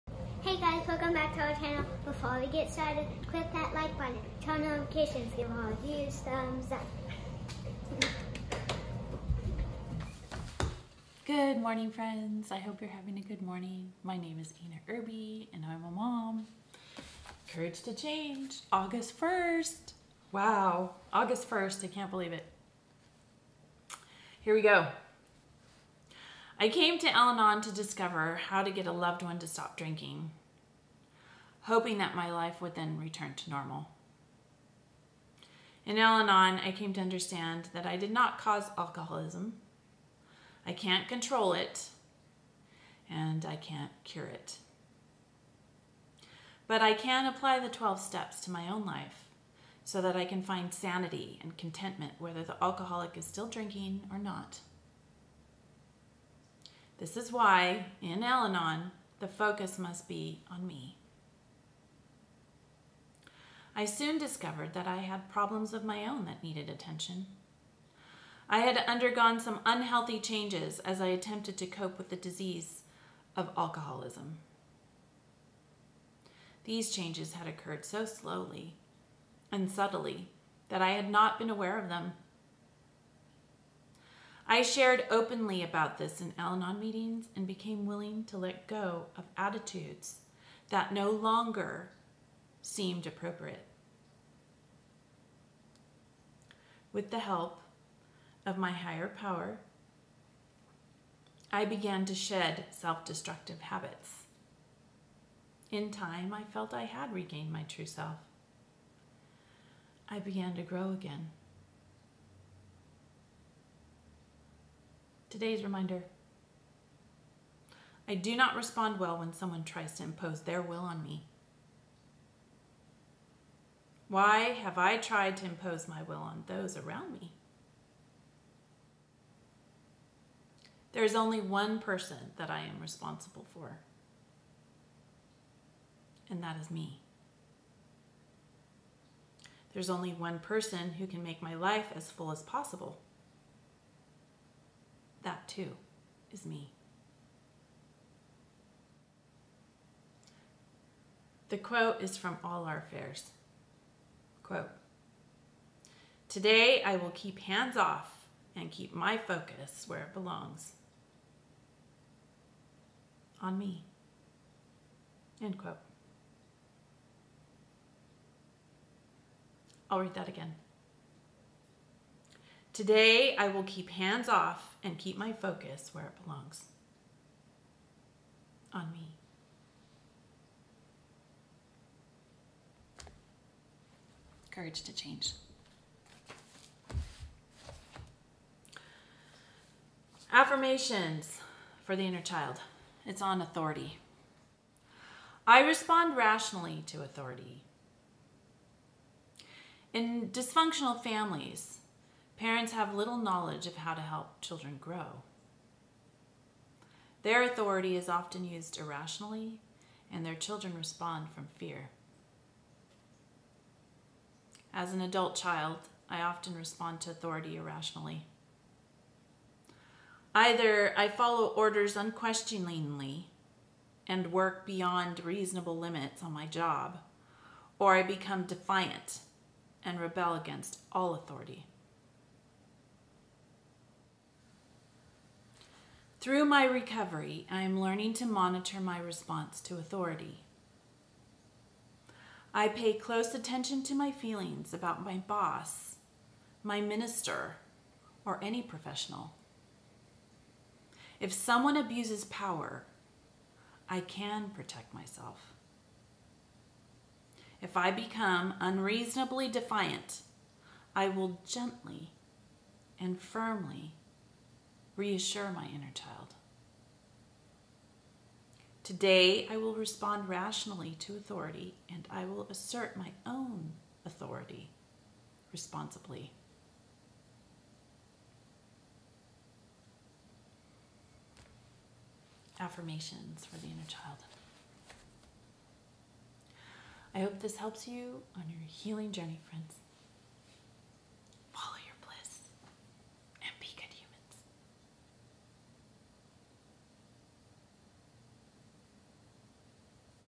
ExJW Courage August 1 2020 I'm Responsible for My Authority/Authority
RECOVERY!!!

It’s been over TEN whole months and NOT destroyed!!!  That’s right even through a pandemic I’m still surviving and thriving on life.  Is it perfect no, but I’m happy.

Did you get that. I’m content with what I have.

I’m REJOICING. I have gotten to read,spend time with my husband,my children,friends, and myself!

Relieved I haven’t gone to ZOOM meetings with bullies for elders and have to listen to hours and hours of talks at a “Always Rejoice” assembly that can now be filtered into my home! Monitored by elders who don’t know my circumstances and who don’t care about me or my family.

I’m so glad I’m recovering and I’m grateful your here to hear me. Keep recovering and don’t go back!

Thank you for coming on my journey!